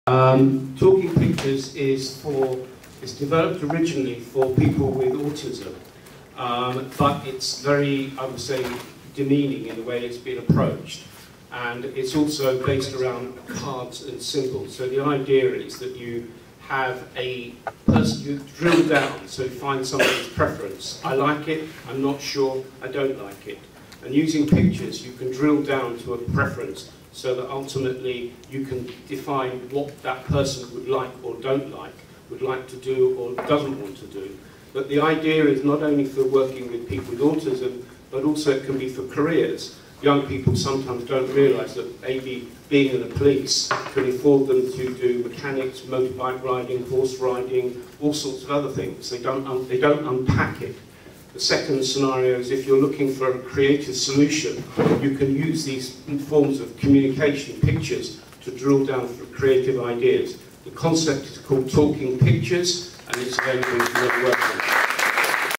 NHSHD #18 Pitch: Talking Pictures
Andrew Cowen presents Talking Pictures; a solution to helping those with Alzheimer's at NHS Hack Day 18: Gloucester.